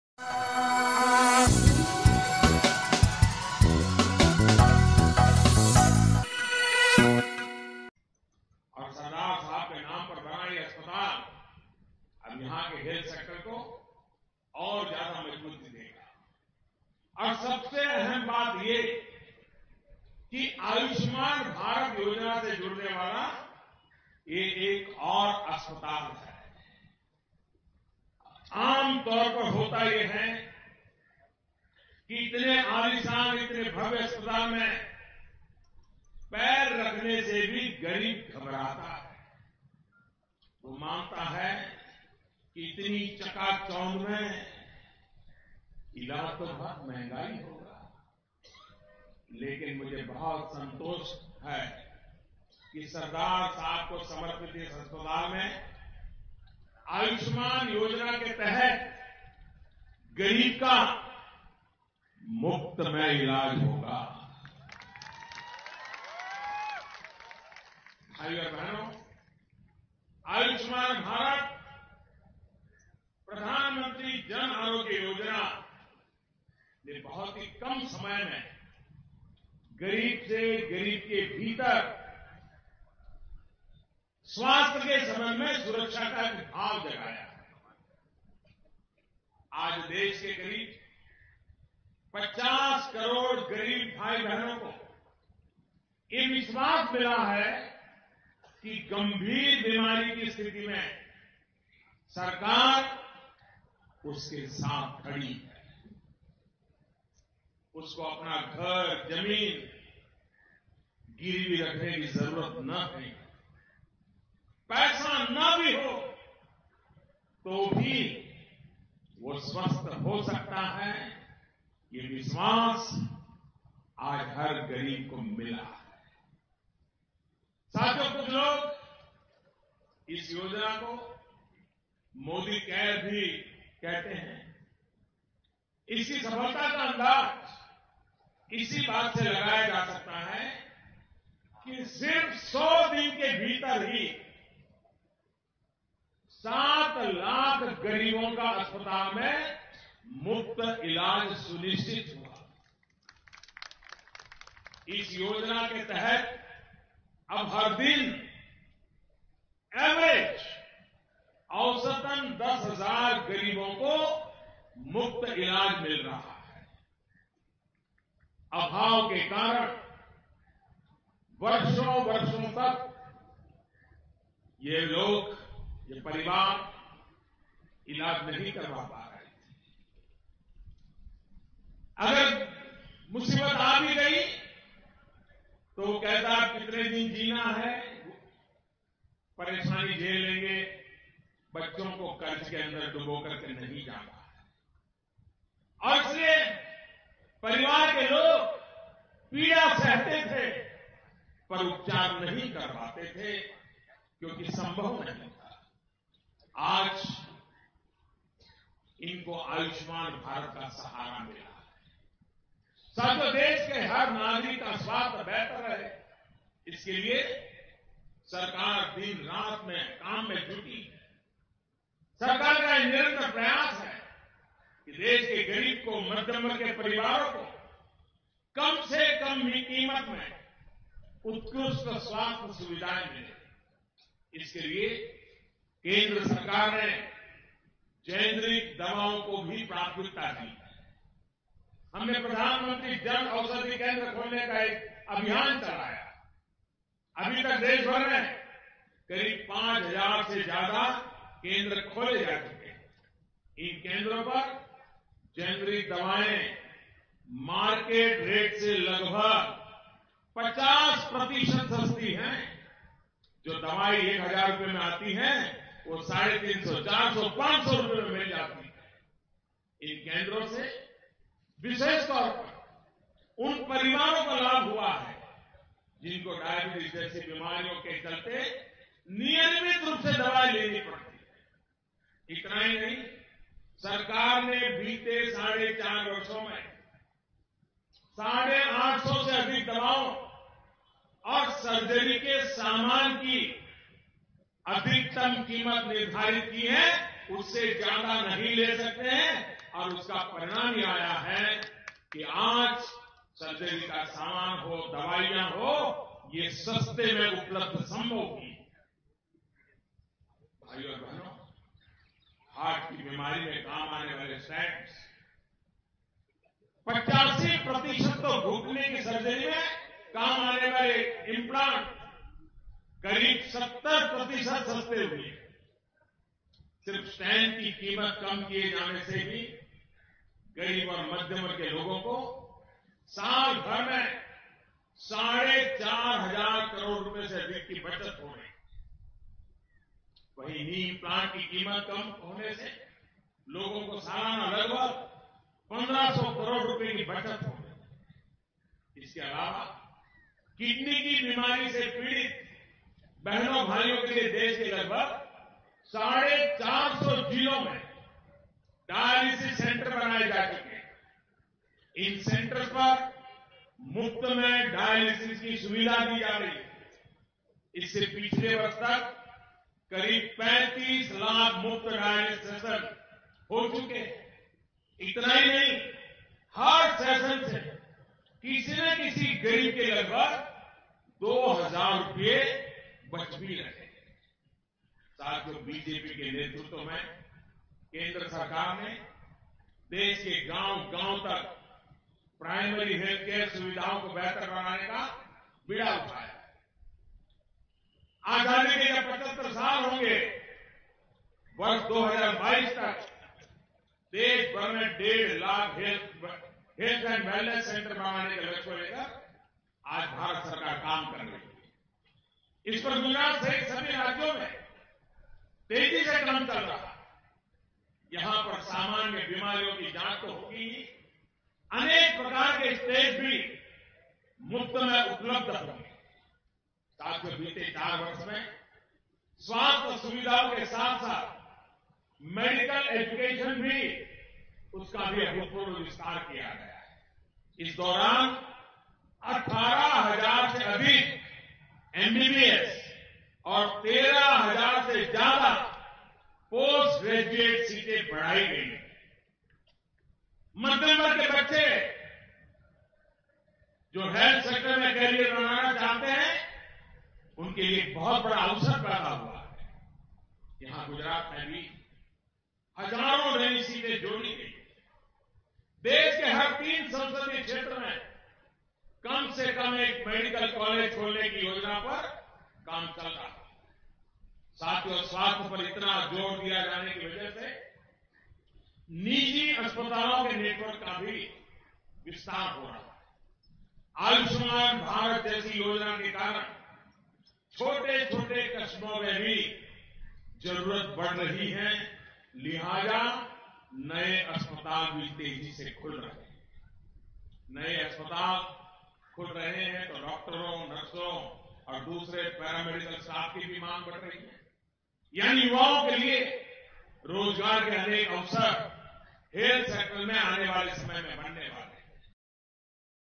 PM Narendra Modi's speech at Inauguration of SVP Hospital in Ahmedabad, Gujarat | Overseas News
PM Narendra Modi's speech at Inauguration of SVP Hospital in Ahmedabad, Gujarat